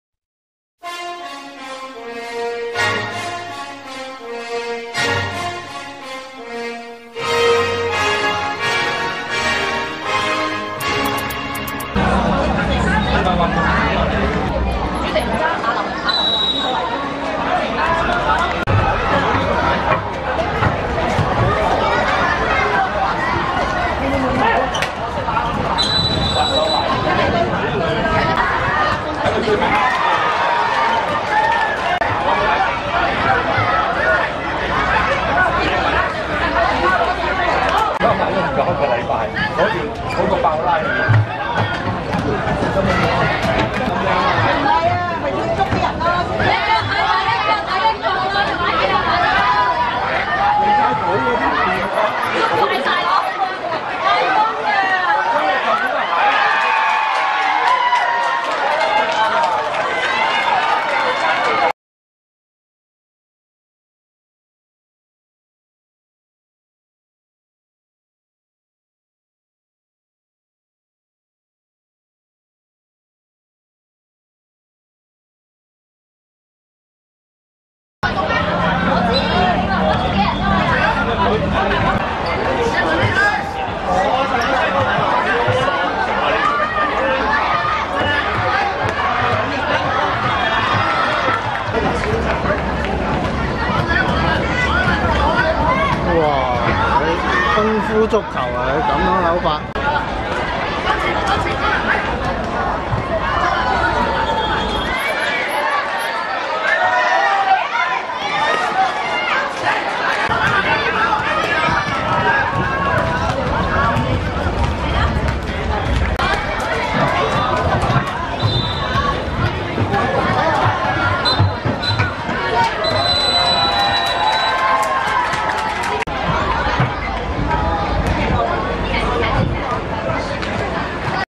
青衣主恩vs英華(2017.5.21.賽馬會五人足球盃~學校組新界南分站U10八強)精華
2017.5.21.興芳路遊樂塲 11:45 青衣主恩(粉紅) 0 : 1 英華(藍)